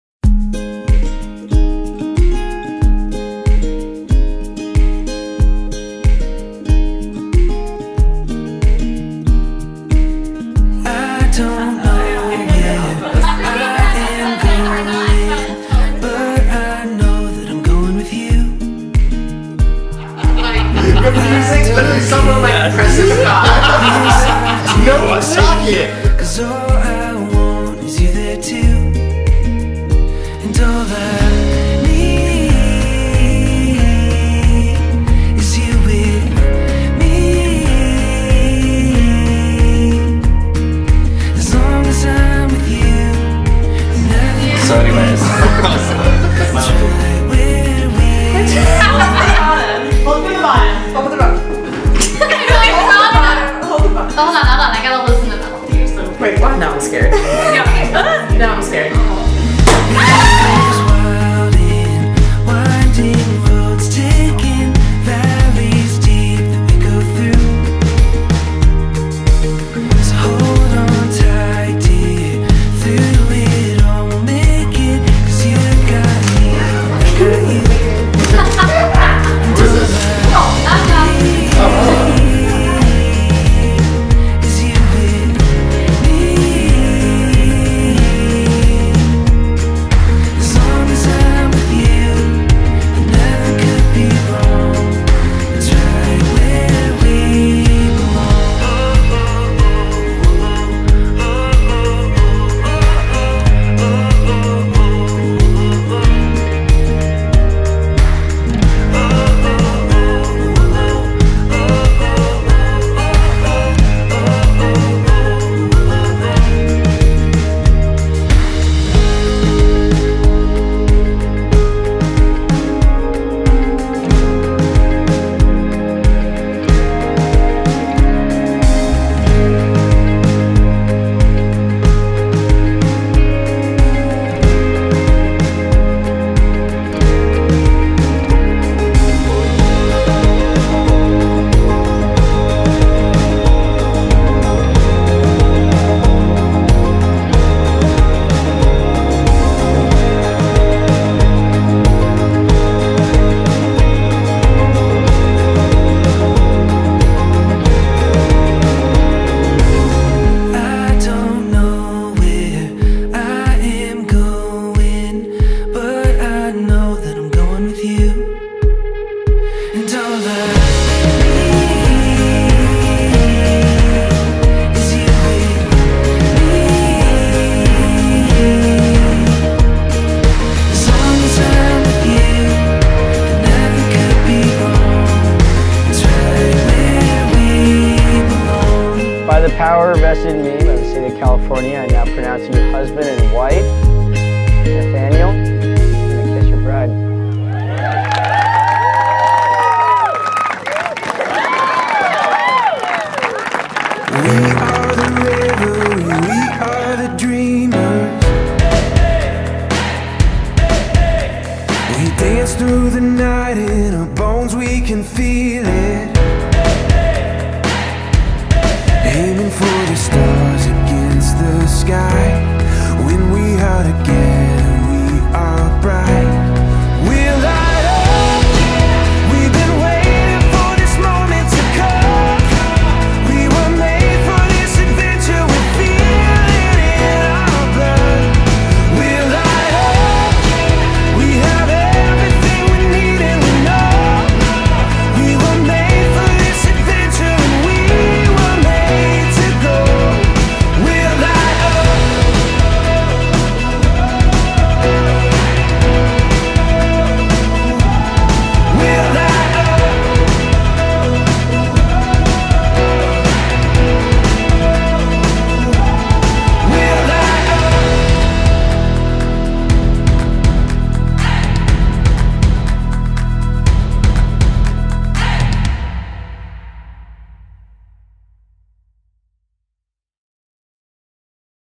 Nate and Chelsea - Oakland Wedding
Nate and Chelsea's Oakland Wedding.

Video by Owl and Tree Wedding Films. Filmed in Oakland, CA. Venue: Morcom Rose Garden